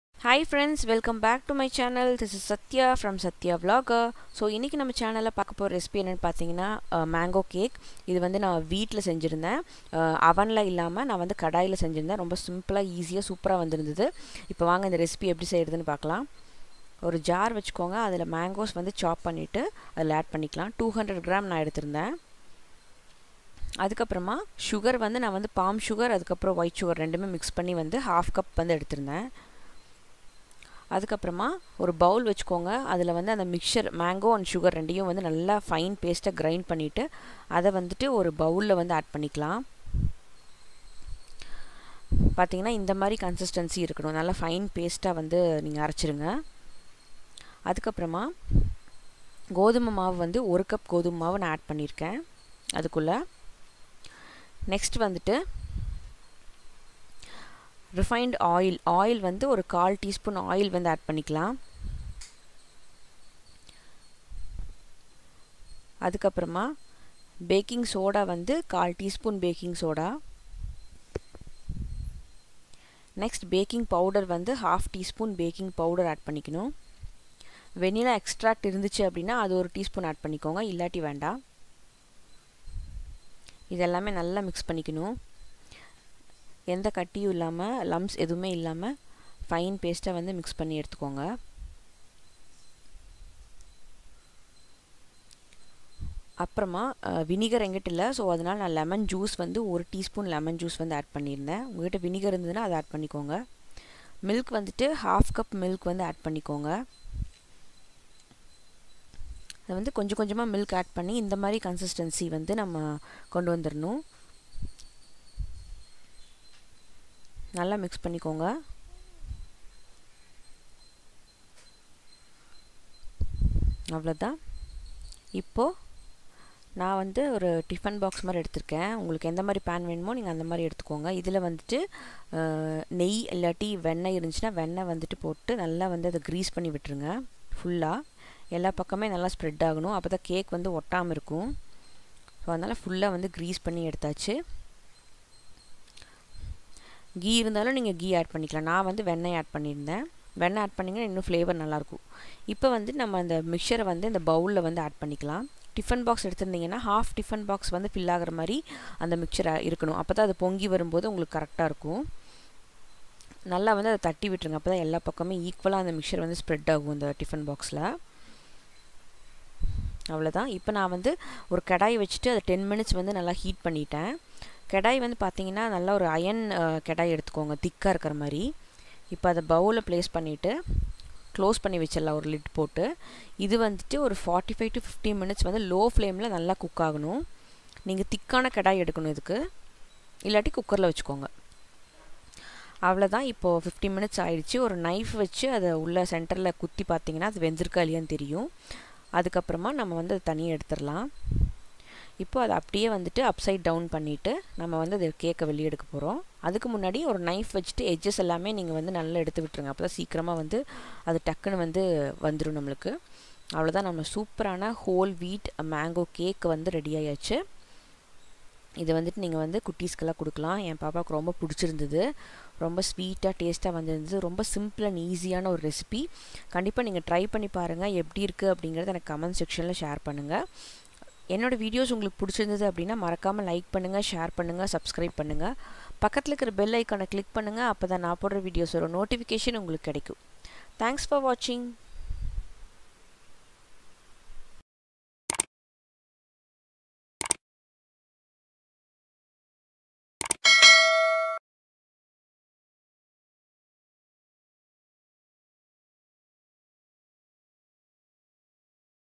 Mango Cake in Kadai with Whole Wheat Flour || Egg-less Mango Cake without Oven, White sugar ,Cream
ingredients and instructions

Chopped Mangoes - 200 gm
Sugar - 1/2 cup(palm sugar)
Grind it into a fine paste

pour it in a bowl
add
wheat flour - 1 cup
Refined oil - 1/4 TSP
Vinegar or lemon juice -  1 TSP

Grease a Bowl
Transfer Mixer into the bowl
preheat the karahi for 10 minutes & place the bowl on it
Cook for 45 - 50 minutes on low flame

yummy mango cake ready to serve

with regards
sathya